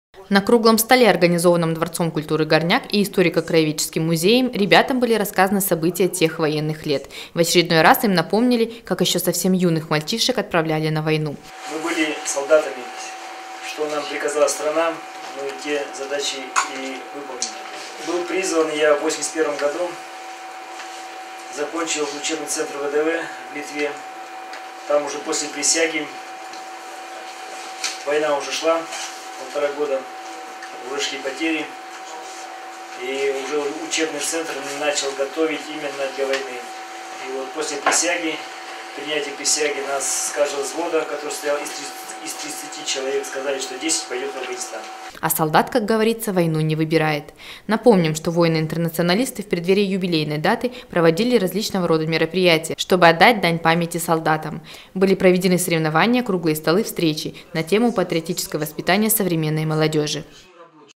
Встреча ветеранов войны в Афганистане с учащимися колледжей
В историко-краеведческом музее состоялась встреча ветеранов войны в Афганистане с учащимися индустриально-технического и горнотехнического колледжей. Мероприятие прошло накануне 30-летнего юбилея вывода Советских войск из Афганистана.

На круглом столе, организованном дворцом культуры «Горняк» и историко-краеведческим музеем ребятам были рассказаны события тех военных лет. В очередной раз им напомнили, как еще совсем юных мальчишек отправляли на войну. А солдат, как говорится, войну не выбирает.

Сергей Баранов, председатель ОО «Союз ветеранов войны в Афганистане г. Степногорска»:
- Мы были солдатами, что приказала страна, все задачи мы выполнили. Я был призван в 1981 году, закончил учебный центр ВДВ в Литве, после присяги, война уже шла 1,5 года, шли потери, учебный центр начал готовить именно для войны. Каждый взвод состоял из 30 человек, 10 из которых направлялись в Афганистан.

Напомним, что воины-интернационалисты в преддверии юбилейной даты, проводили различного рода мероприятия, чтобы отдать дань памяти солдатам. Были проведены соревнования, круглые столы, встречи на тему: «Патриотическое воспитание современной молодежи».